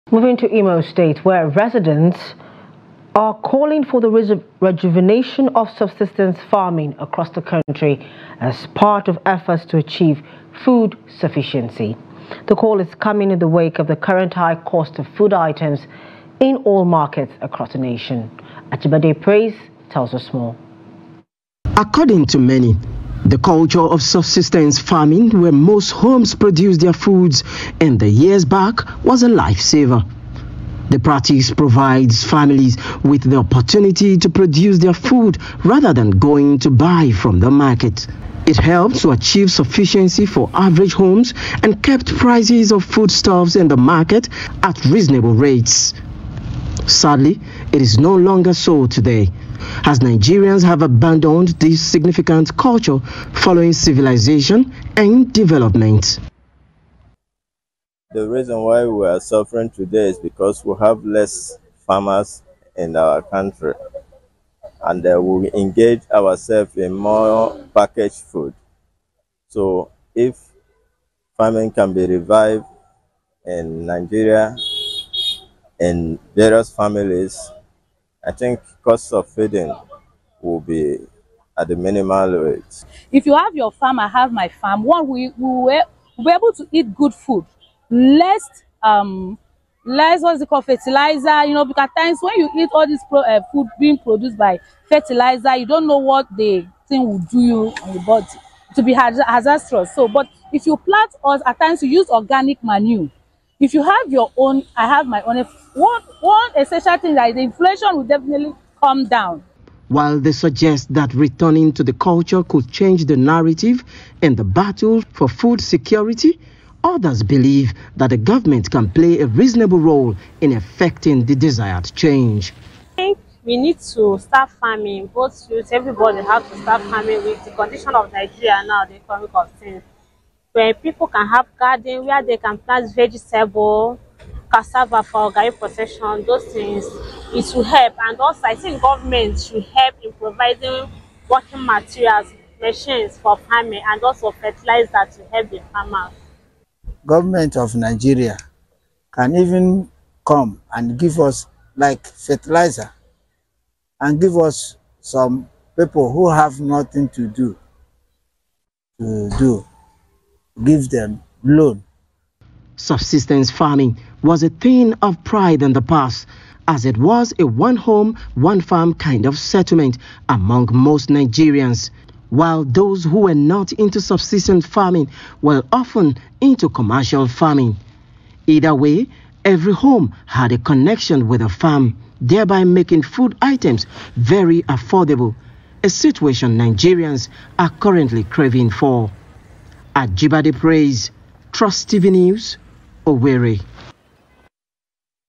Imo Residents Call For Rejuvenation Of Subsistence Farming
Residents of Imo state in southeast Nigeria are calling for the rejuvenation of subsistence farming across the country as part of efforts to achieve good sufficiency.

The call is coming in the wake of the current high cost of food items in all markets across the nation.